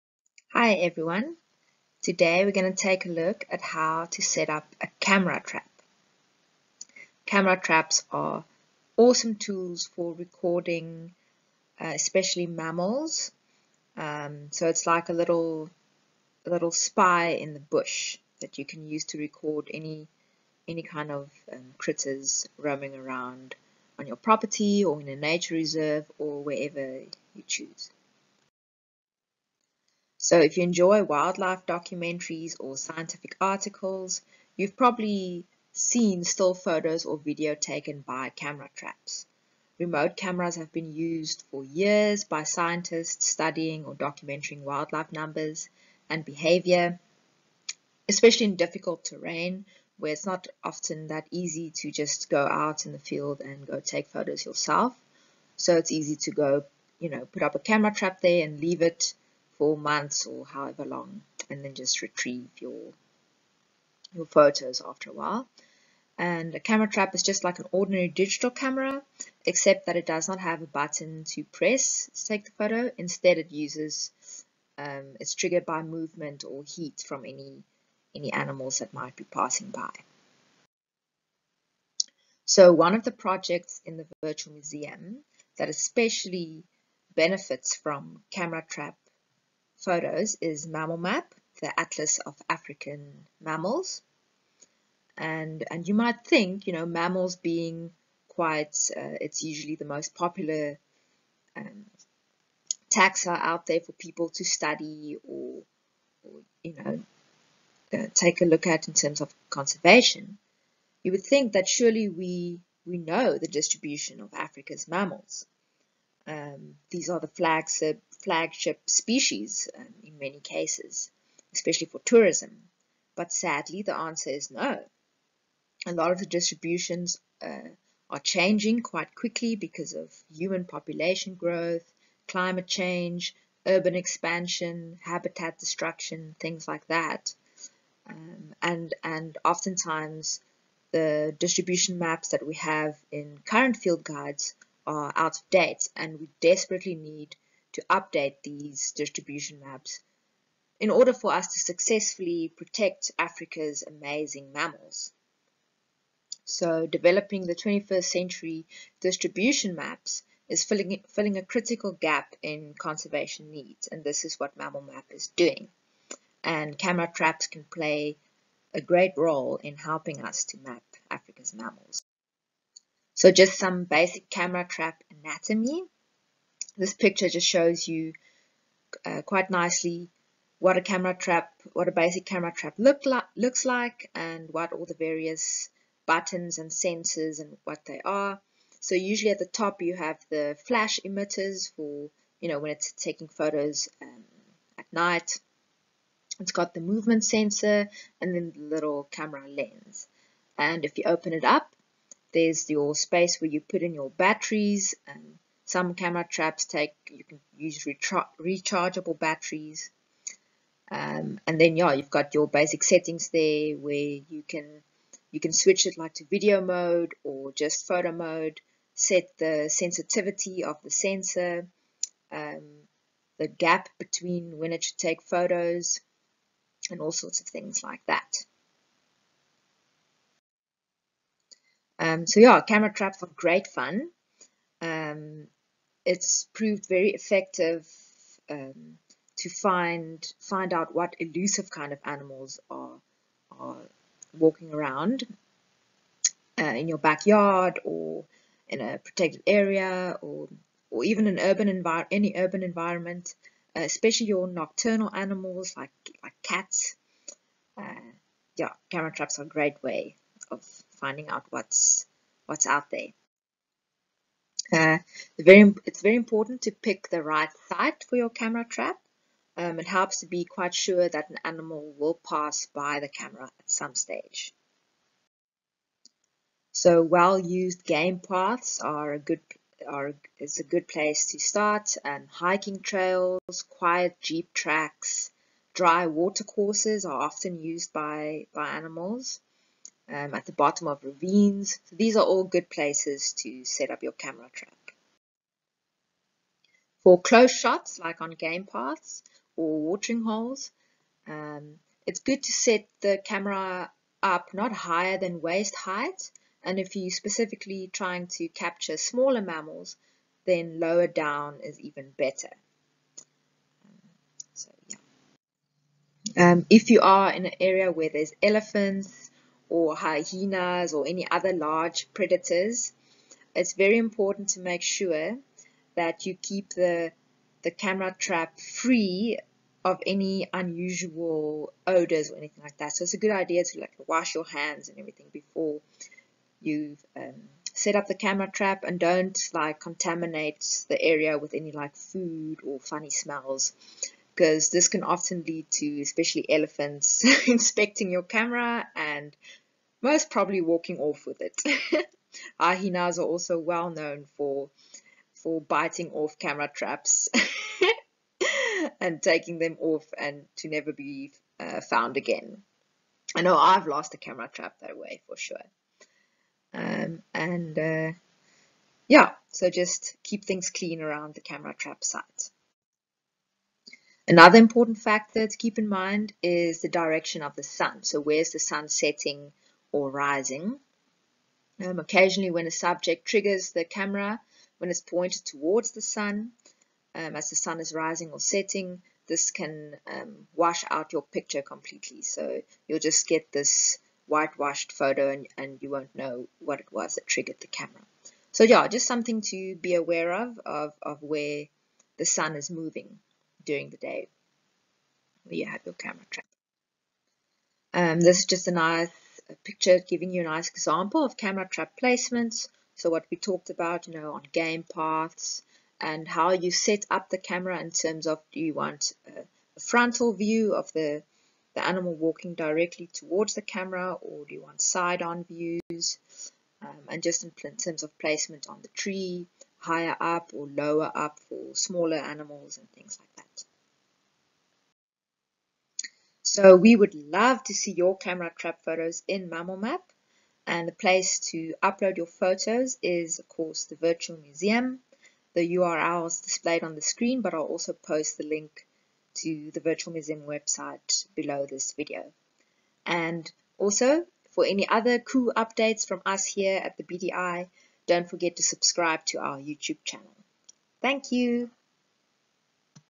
How to set up a camera trap - MammalMAPping
Megan Loftie-Eaton talks about camera trapping and mammal mapping. Camera traps are great fun! Camera-trapping has proved to be a very effective way of finding out which elusive and, especially, nocturnal animals are in an area or on your property.

MammalMAP is the Atlas of African Mammals. The aim of MammalMAP is to update the distribution records for all of Africa’s wild mammals – the small ones, the big ones, the dry ones and the wet ones

“Surely we know the distributions of Africa’s mammals? These are flagships species for tourism in Africa.” Sadly, the answer is “No” – the distributions are changing due to habitat destruction and climate change. Developing the 21st century distribution maps is filling a critical gap in conservation needs.